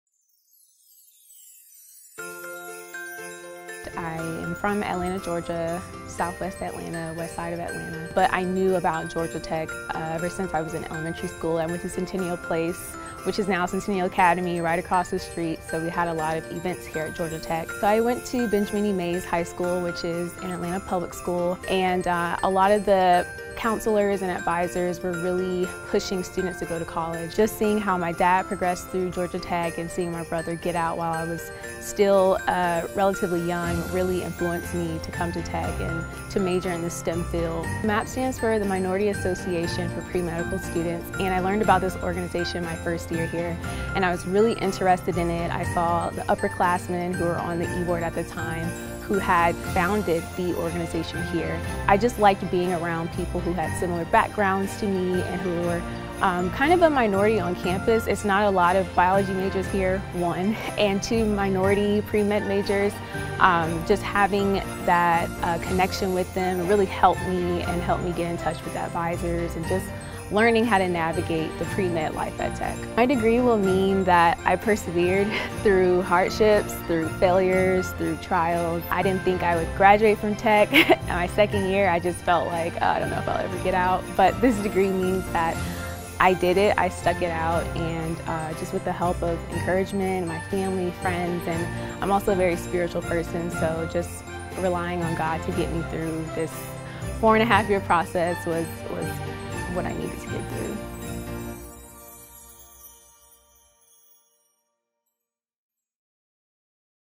Georgia Tech 2018 Graduate: Erica Peek
Atlanta native and Mays High School graduate Erica Peek will follow in her father and brother's footsteps earning her undergraduate degree from Georgia Tech. Peek's Biology degree will eventually lead to medical school.